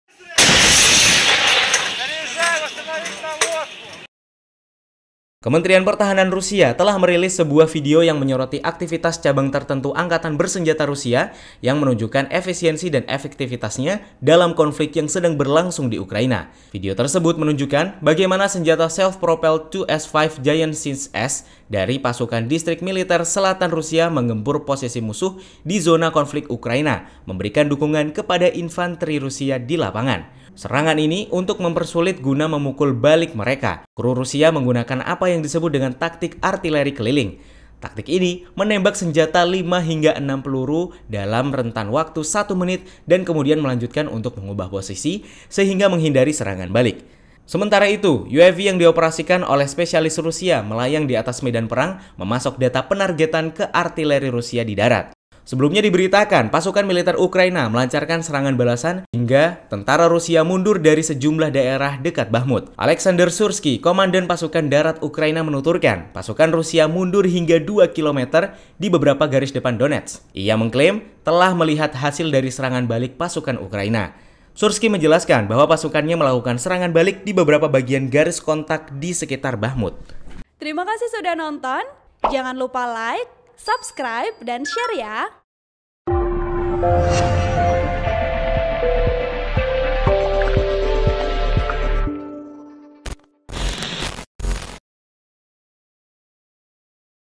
Militer Rusia Gunakan Artileri Self Propelled untuk Pukul Mundur Tentara Militan Ukraina
TRIBUN-VIDEO.COM - Kementerian Pertahanan Rusia telah merilis sebuah video yang menyoroti aktivitas cabang tertentu Angkatan Bersenjata Rusia yang menunjukkan efisiensi dan efektivitasnya dalam konflik yang sedang berlangsung di Ukraina.

Video tersebut menunjukkan bagaimana senjata self-propelled 2S5 Giatsint-S dari pasukan Distrik Militer Selatan Rusia menggempur posisi musuh di zona konflik Ukraina, memberikan dukungan kepada infanteri Rusia di lapangan.

Untuk mempersulit musuh untuk memukul balik mereka, kru Rusia menggunakan apa yang disebut taktik "artileri keliling".

Taktik ini menembakan senjata lima hingga enam peluru dalam rentang satu menit dan kemudian melanjutkan untuk mengubah posisi, sehingga menghindari serangan balik.

Sementara itu, UAV yang dioperasikan oleh spesialis Rusia melayang di atas medan perang, memasok data penargetan ke artileri Rusia di darat.

Sebelumnya diberitakan pasukan Militer Ukraina melancarkan serangan balasan, hingga tentara Rusia mundur dari sejumlah daerah dekat Bakhmut.

Oleksandr Syrskyi Komandan pasukan darat Ukraina menuturkan, pasukan Rusia mundur hingga dua kilometer di beberapa garis depan Donetsk.

Ia mengklaim telah melihat hasil dari serangan balik pasukan Ukraina.

"Kami melihat hasil tindakan efektif unit kami," ujar Syrskyi

Syrskyi menjelaskan bahwa pasukannya melakukan serangan balik di beberapa bagian garis kontak di sekitar Bakhmut.

(TribunVideo.com/Tribunnews)

Artikel ini telah tayang di Kompas.com dengan judul "Serangan Balik Ukraina, Rusia Mundur dari Dekat Bakhmut", Klik untuk baca
Editor : Aditya Jaya Iswara

Host: Abdul Salim
Vp: Fajar